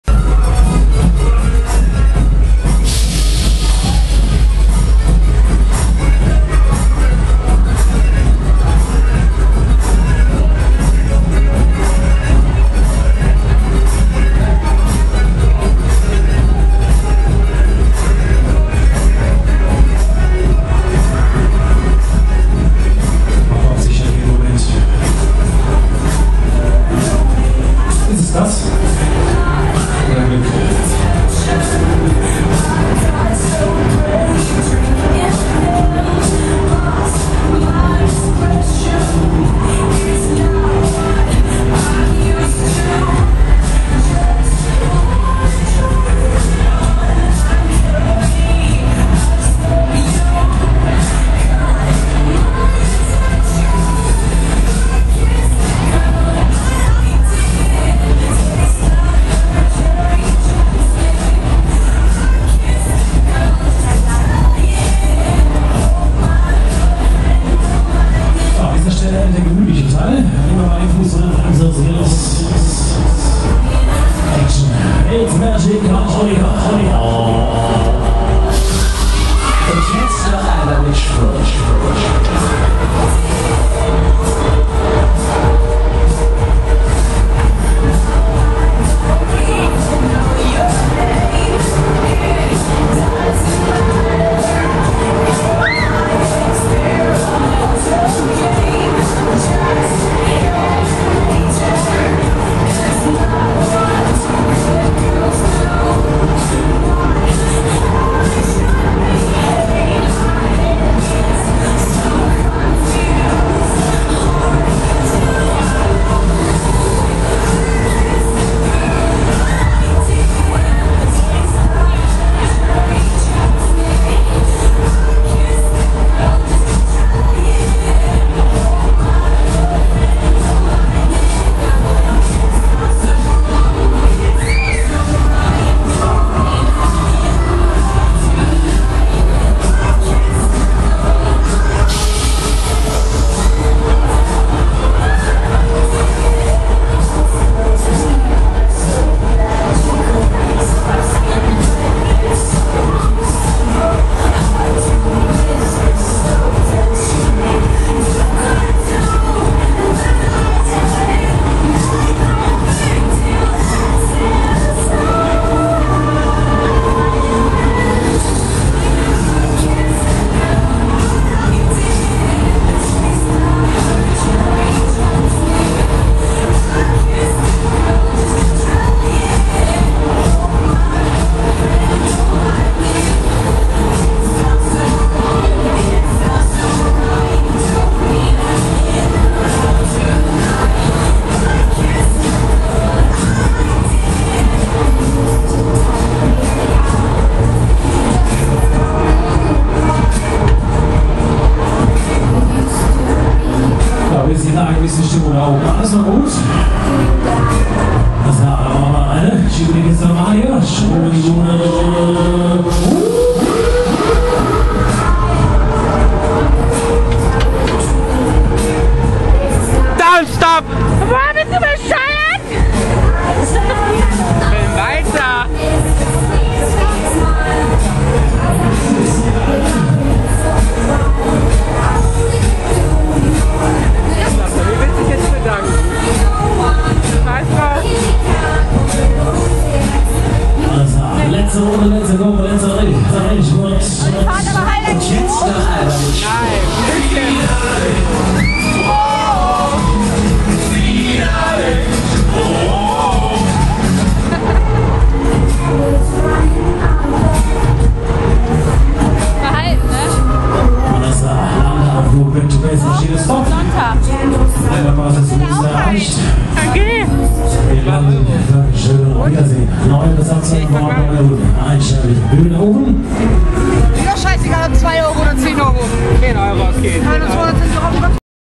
Circus Circus (Offride) in Münster 2010
Bruch Circus Circus (Offride) am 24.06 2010 in Münster auf dem Sommersend /  Muenster Send